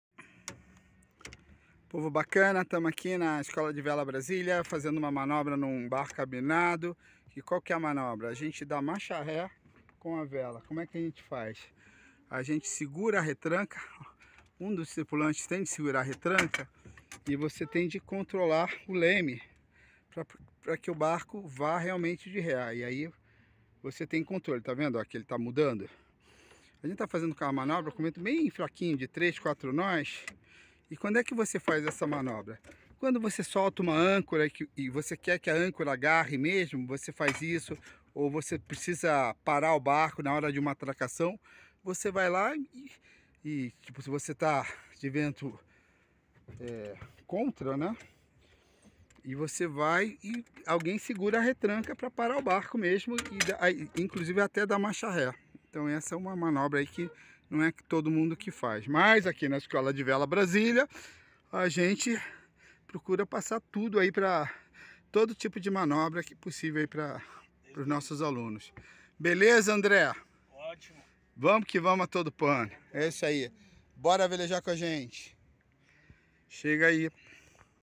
Marcha ré com as velas - Escola de Vela Brasília
Manobra necessária para ajudar a unhar âncora, ou em uma atracação onde seja necessário 'freiar' o barco